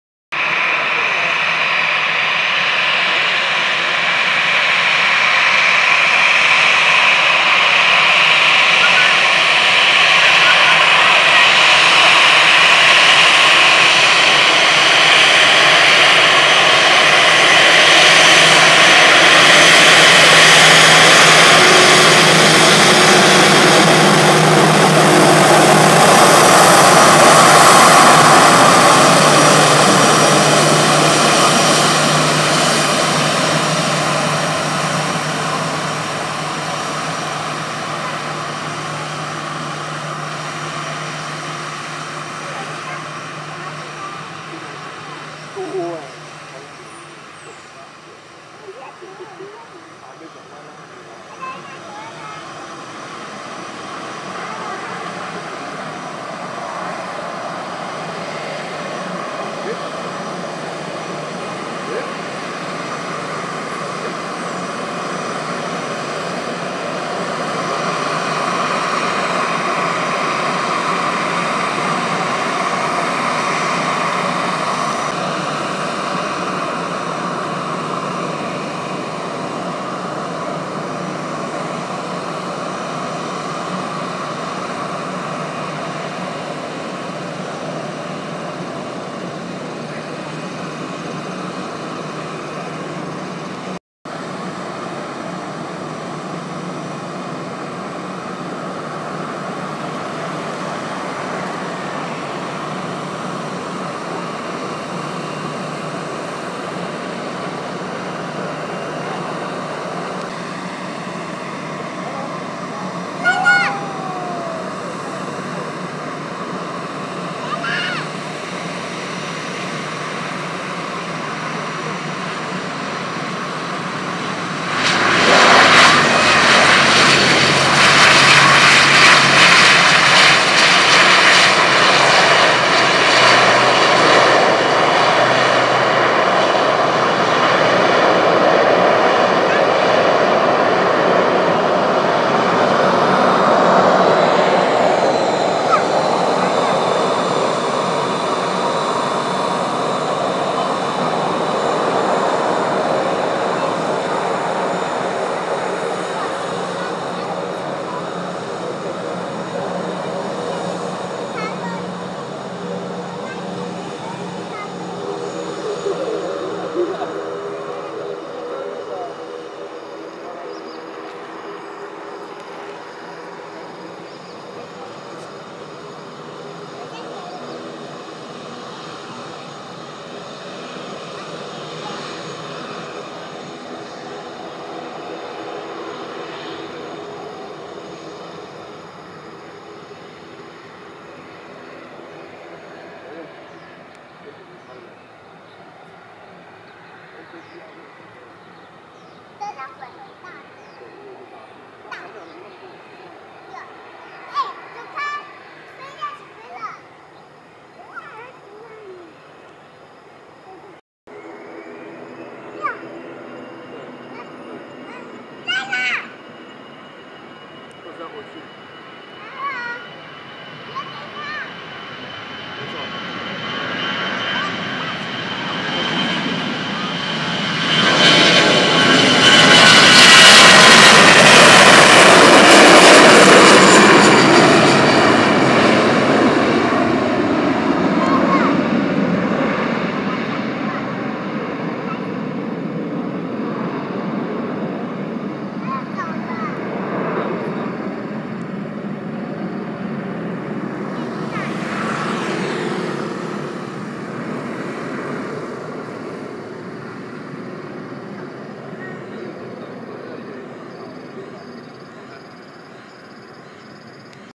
【JL尼歐】金門機場 長榮航空 EVA AIR Airbus A321 (B-16227) 等待華信航空 ATR 72-600 (B-16851) 降落後起飛 CLOSE UP
【JL尼歐】
長榮航空 EVA AIR Airbus A321 (B-16227)
等待華信航空 ATR 72-600 (B-16851) 降落後起飛

EVA AIR Airbus A321 (B-16227) is waiting for take off while Mandarin Airlines ATR 72-600 (B-16851) is approaching for landing at Kinmen Airport.